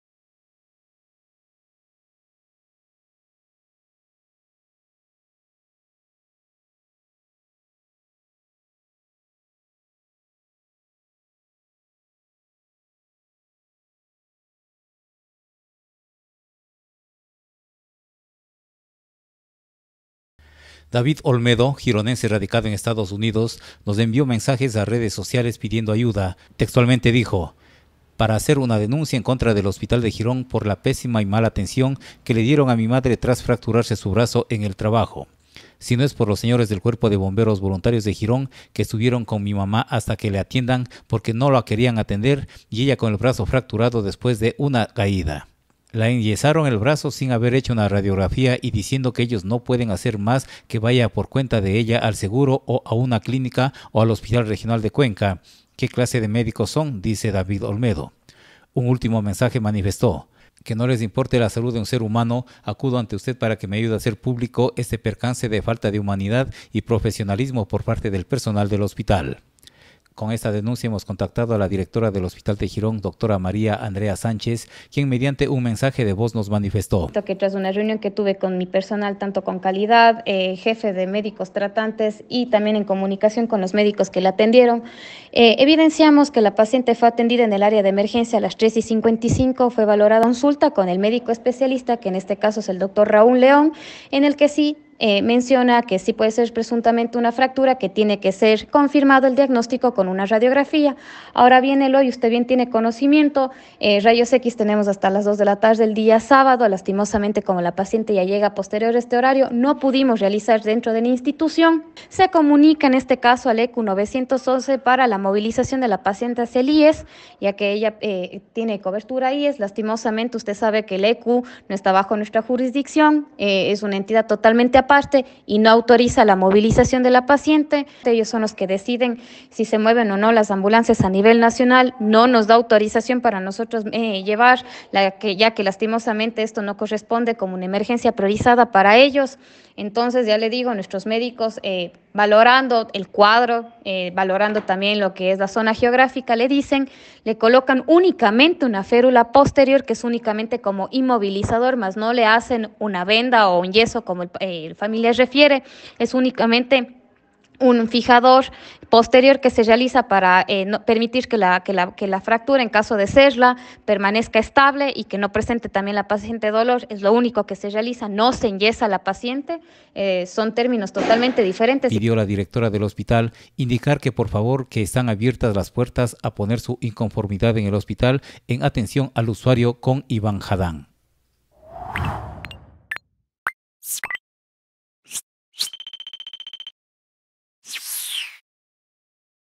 Denuncian mala atención a una paciente adulta mayor
A este medio de comunicación, un ciudadano residente en EEUU ha hecho llegar una denuncia manifestando que su madre no fue atendida correctamente en el hospital de Girón el 21 de septiembre ante una posible fractura. De su lado la directora de la casa de salud gironense explicó la situación, manifestando que se cumplió con la atención requerida pero, el problema surge por el uso de ambulancia, la cual debe ser autorizada desde el ECU911.